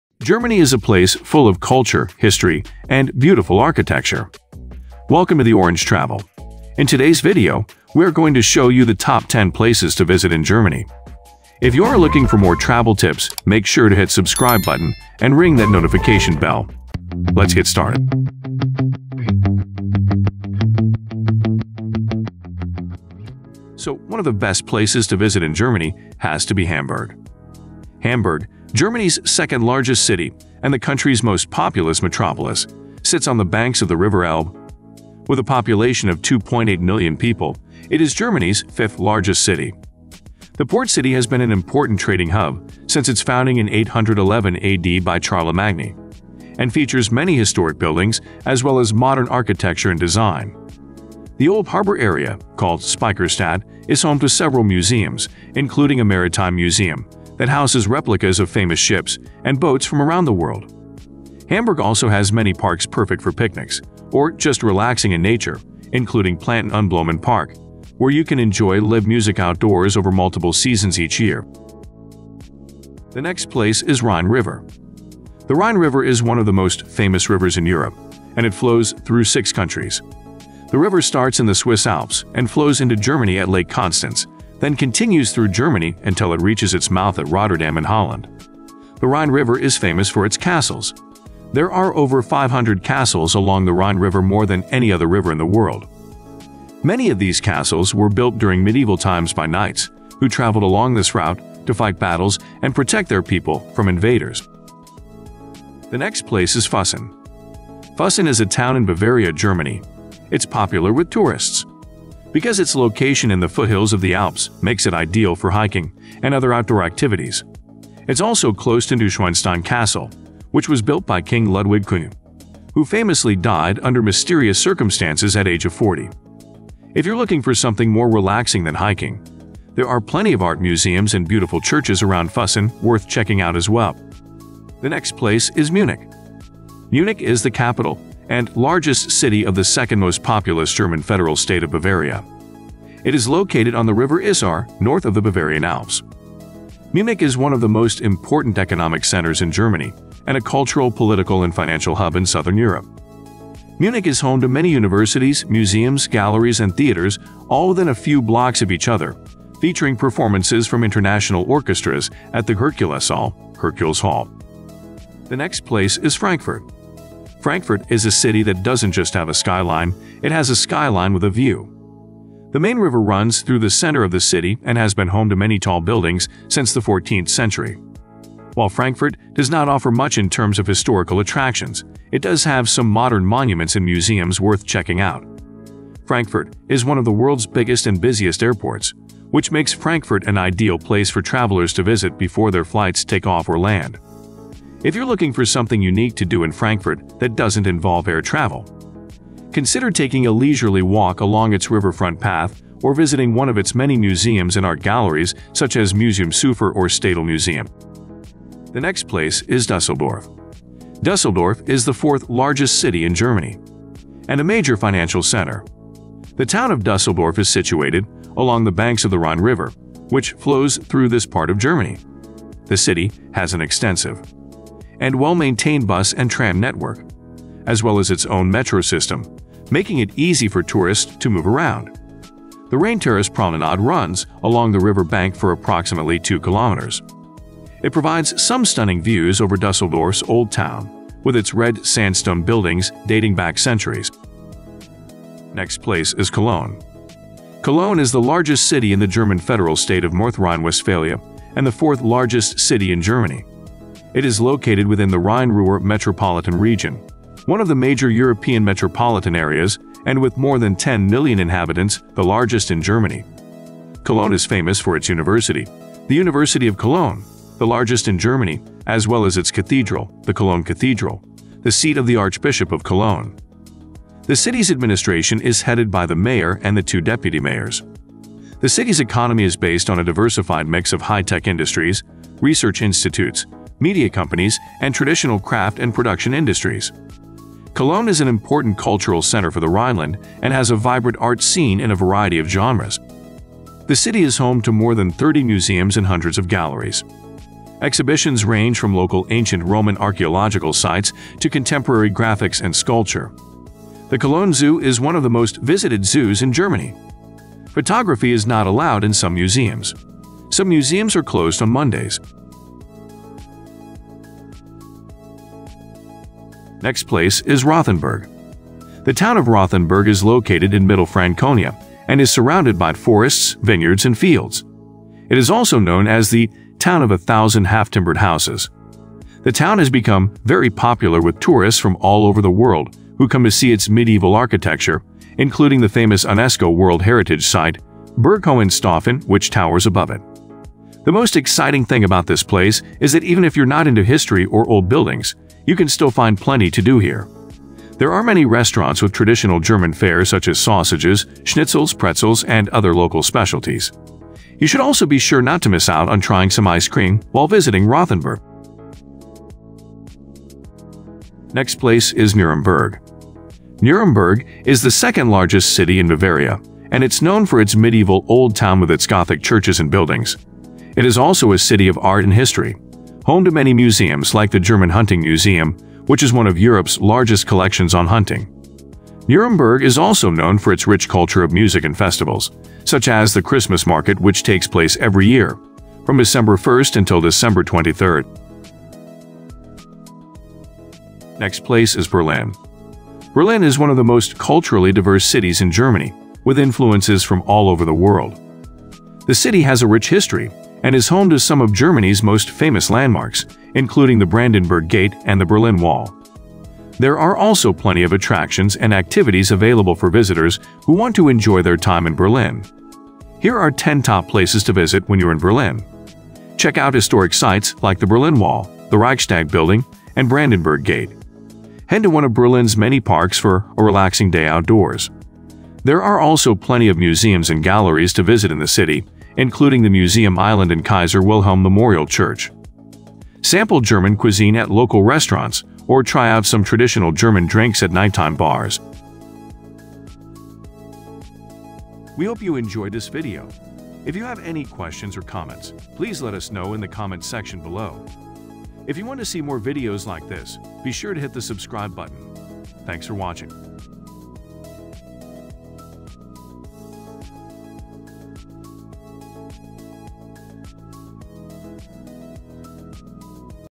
Top 10 Places To Visit In Germany - Travel Guide
Germany is a beautiful country with a lot to offer. From its stunning scenery to its rich culture and history, there is something for everyone in Germany.

If you're planning a trip to Germany, then be sure to check out this video. I have listed the top 10 places to visit in Germany with travel guide. These are the must-see attractions in Germany that you won't want to miss.

So, if you're looking for some inspiration for your next trip, then be sure to watch this video. I'm sure you'll find something that interests you.

Note:
Every footage used in this video is under fair usage.